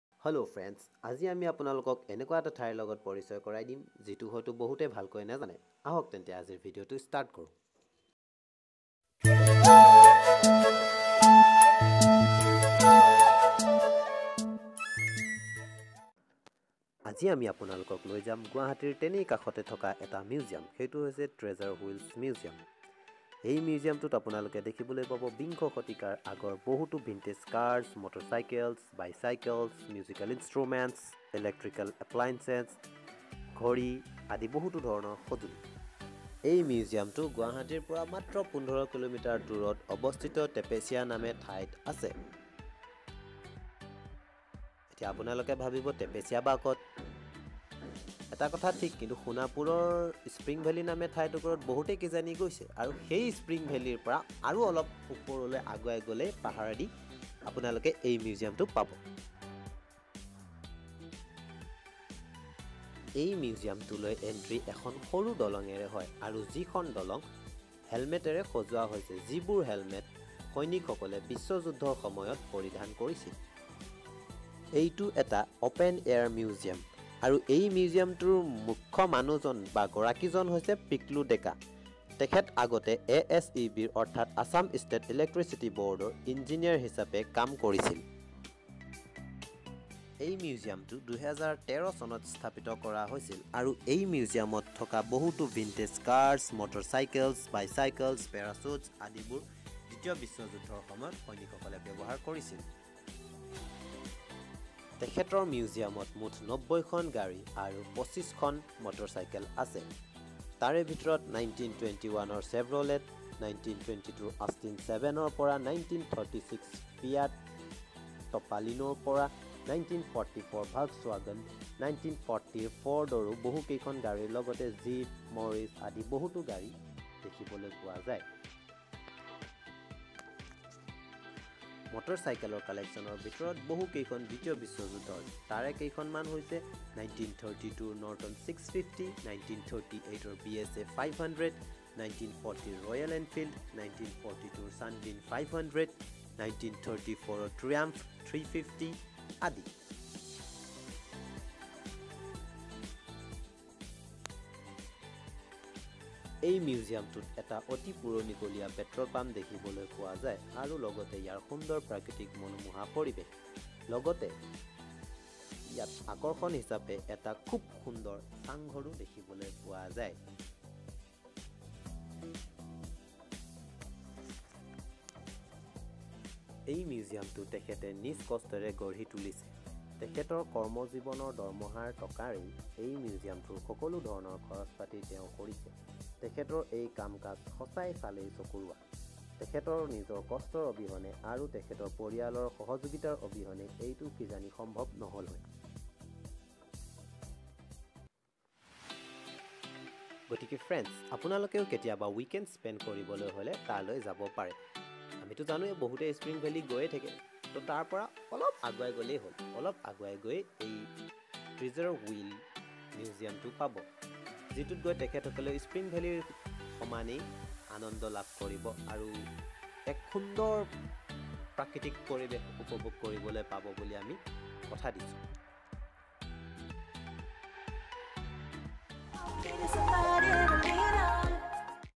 Treasured Wheels Museum || Tepesia Sonapur Guwahati
Please do visit Treasured Wheels Museum. Its a very naturally beautified open air museum. There is lots of vintage cars, bikes, bicycles, musical instruments ect.
Owner of this museum is Pikloo Deka, he is retired engineer of ASEB.For more info about his and  this museum then please do visit or comment in the comment section.

Note: Some sources collected from his official facebook page.